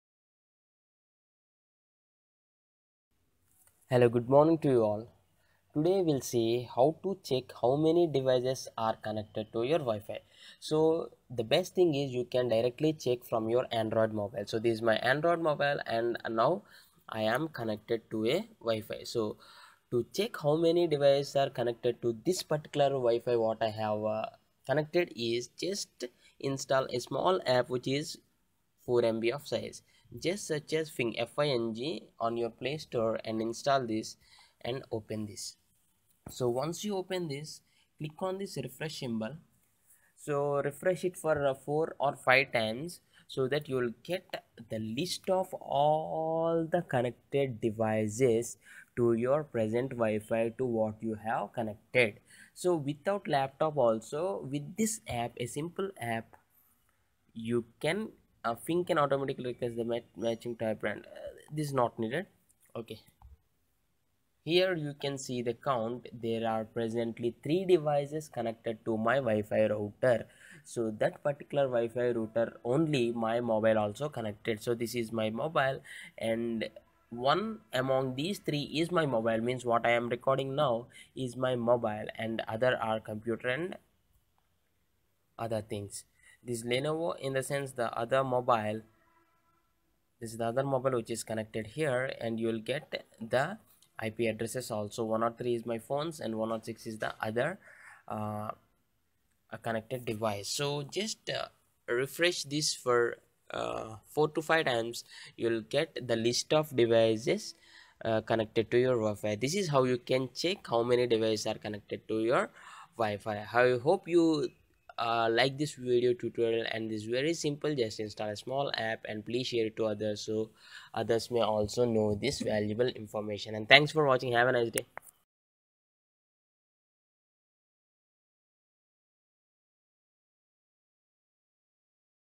how to check how many devices are connected to your wifi
how to check how many devices are connected to your wifi,yes we can check the number of devices connected with their names too for example lenovo mobile,some user on laptop is connected to your present using wifi.